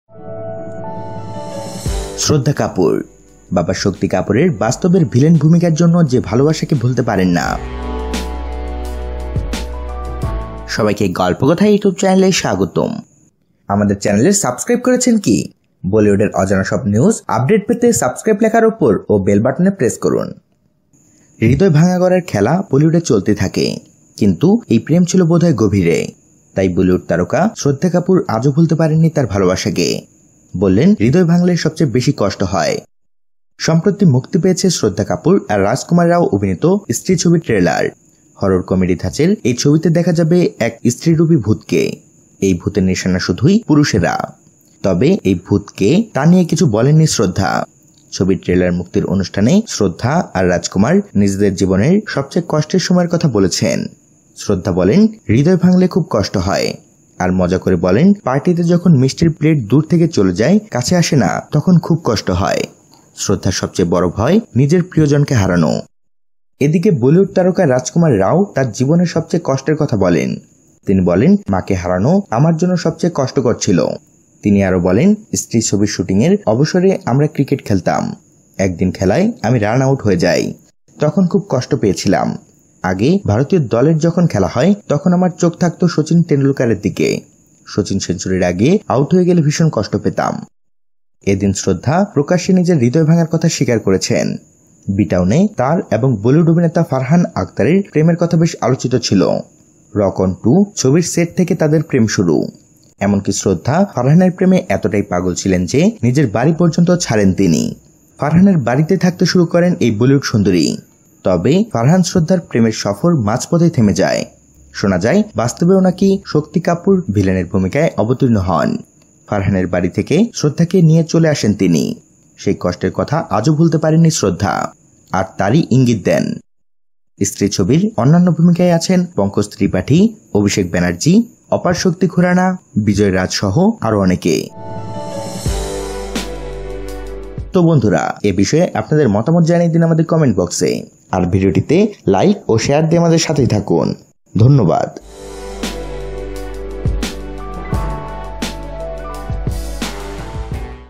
শ্রদ্ধা কাপুর | বাবা শক্তি কাপুরের বাস্তবের ভিলেন ভূমিকার জন্য যে ভালোবাসাকে ভুলতে পারেন না
♥ Questions? হৃদয় ভাঙা-গড়ার খেলা বলিউডে চলতেই থাকে। কিন্তু এই প্রেম ছিল বোধ হয় গভীরে। তাই বলিউড তারকা শ্রদ্ধা কাপুর আজও ভুলতে পারেননি তাঁর ভালোবাসাকে। বললেন, ‘হৃদয় ভাঙলে সবচেয়ে বেশি কষ্ট হয়।’